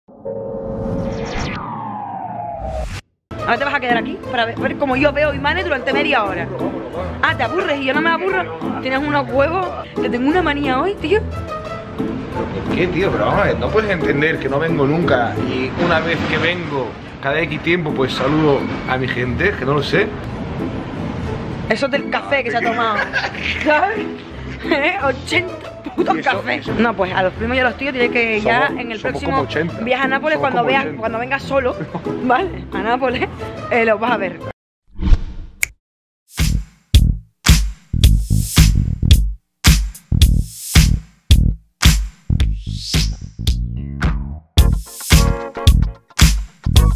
Marta Peñate y Tony Spina viajan a Nápoles y terminan muy enfadados | mtmad
Marta Peñate regresa una semana más a su canal de mtmad para ponernos al día de todas las novedades que hay en su vida. Después de criticar sin piedad las publicidades que hacen algunos de sus compañeros, la exconcursante de ‘La isla de las tentaciones’ ha cogido la maleta y un avión para viajar junto a Tony Spina a Nápoles. Los futuros marido y mujer están disfrutando de unas vacaciones en un maravilloso hotel con una habitación con vistas al mar. Pero no solo nos han enseñado donde duermen, sino que nos han llevado a dar una vuelta para mostrárnoslo todo. Sin embargo, no todo ha salido tan bien como esperaban, y por eso Marta está de mal humor. El buen tiempo no les ha acompañado y han tenido algún que otro percance.

La vida de influencer es muy dura y sino, que se lo digan a Marta. La que fuera concursante de ‘Gran hermano’ quería que su chico le hiciera una buenas fotos para sus redes sociales y esto ha sido el motivo por el que la pareja ha tenido un encontronazo. Menos mal que poco después han visitado una iglesia muy famosa de la ciudad donde podrían casarse. Como buen italiano, Tony le ha hecho de traductor y guía turístico a su futura esposa, pero poco les ha durado la felicidad, ya que a Tony no le ha gustado nada lo que Marta ha opinado sobre su ciudad.